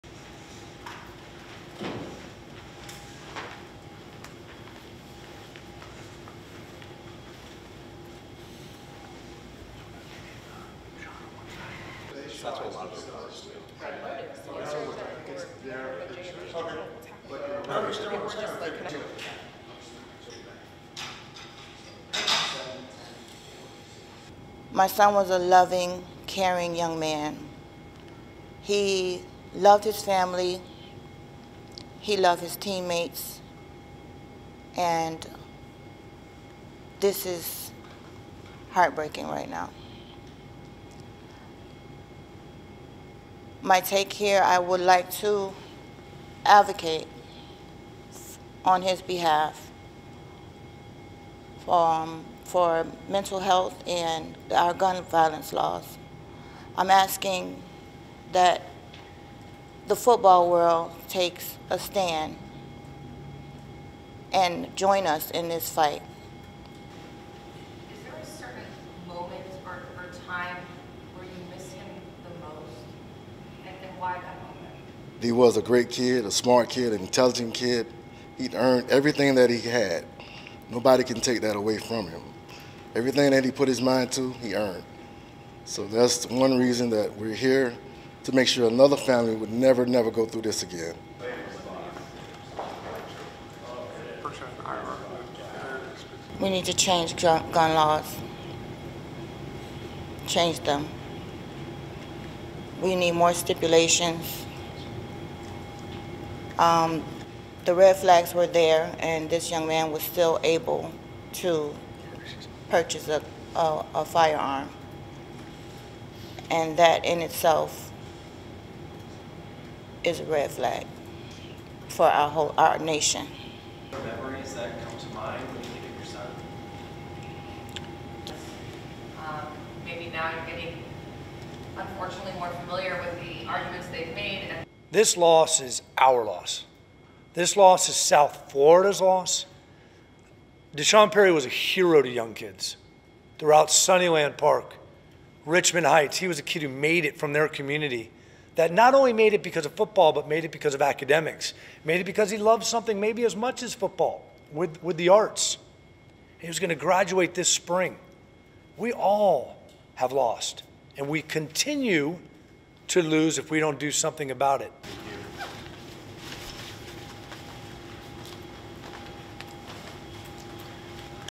Parents of slain UVA athlete call for gun control
(15 Dec 2022) ++SNTV clients may use++

RESTRICTION SUMMARY:
ASSOCIATED PRESS
Coral Gables, Florida – 15 December 2022
1. Perry family and lawyers arriving for press conference
2. Posters
3. Closeup poster
4. SOUNDBITE (English) Happy Perry, mother of D'Sean Perry, UVA student-athlete killed in a mass shooting:
"My son was a loving, caring young man. He loved his family. He loved his teammates. And. This is heartbreaking right now. My take here, I would like to advocate on his behalf. For for mental health and gun violence laws. I'm asking that the football world takes a stand. And join us in this fight."
5. Wide press conference
6. SOUNDBITE (English) Sean Perry, father of D'Sean Perry, UVA student-athlete killed in a mass shooting:
"He was a great kid, a smart kid, an intelligent kid. He'd earned everything that he had. Nobody can take that away from him. Everything that he put his mind to, he earned. So that's one reason that we're here, to make sure another family would never, never go through this again."
7. Poster
8. SOUNDBITE (English) Happy Perry, mother of D'Sean Perry, UVA student-athlete killed in a mass shooting:
"We need to change gun laws. Change them. We need more stipulations. The red flags were there and this young man (the shooter) was still able to purchase a firearm. And that in itself is a red flag for our whole our nation."
9. Closeup UVA colors
10. Wide
11. SOUNDBITE (English) Michael Haggard, lawyer with Haggard law firm:
"This loss is our loss. This loss is South Florida's loss. D'Sean Perry was a hero to young kids throughout Suniland Park, Richmond Heights. He was a kid who made it from their community that not only made it because of football but made it because of academics. Maybe because he loved something maybe as much as football with the arts. He was going to graduate this spring. We all have lost, and we continue to lose if we don't do something about it."
12. Family leaving press conference
STORYLINE:
The parents of a University of Virginia football player killed in a shooting last month called Thursday for stricter gun laws and more mental health support while asking the larger world of college football to stand with them.
"My son was a loving, caring young man," said Happy Perry, the mother of D'Sean Perry. "He loved his family. He loved his teammates. And this is heartbreaking right now. I would like to advocate on his behalf for mental health and our gun violence laws."
She added: "I'm asking that the football world takes a stand and join us in this fight."
Perry and teammates Lavel Davis Jr. and Devin Chandler were killed Nov. 13 on a charter bus that carrying University of Virginia students back to Charlottesville from a field trip to the nation's capital.
Christopher Darnell Jones Jr., 23, a fellow student and former football player, opened fire as the bus returned to campus, police said. Two other students, football player Mike Hollins and student Marlee Morgan, were wounded in the shooting.
The killings come as the nation recoils from a string of mass shootings over six months. Those included an attack that killed 19 students and two teachers at an Uvalde, Texas, elementary school; a shooting at a Fourth of July parade in a Chicago suburb that killed seven and wounded more than 30; and a shooting at a Buffalo, New York, supermarket that killed 10 people and wounded three.